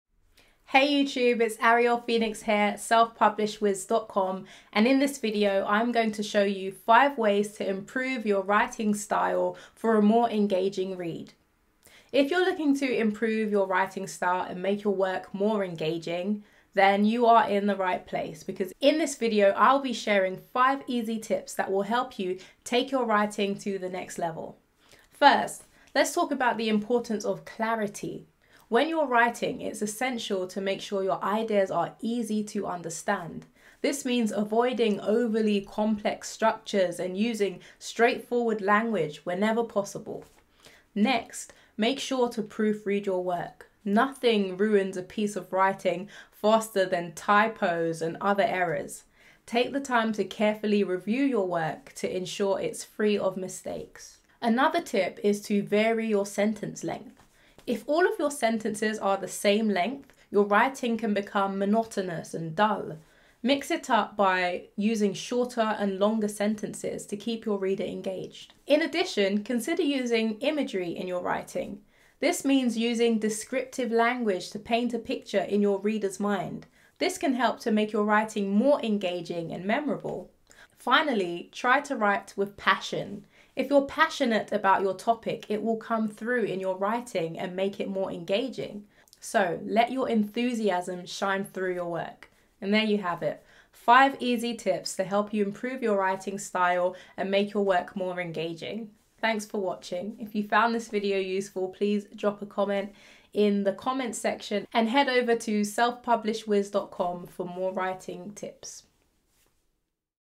5 Ways to Improve Your Writing Style for A More Engaging Read
Are you looking for ways to make your writing more appealing to your readers? In this video, I'll go over 5 tips for improving your writing style and creating a more compelling read.

By following these tips, you can improve your writing style and create a more engaging and captivating read for your audience.